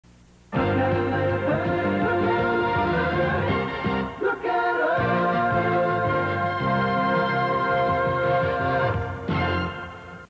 WPXI (NBC) Station ID 1984 "Look at 11"
This is WPXI's station ID from 1984
WPXI is located in Pittsburgh, PA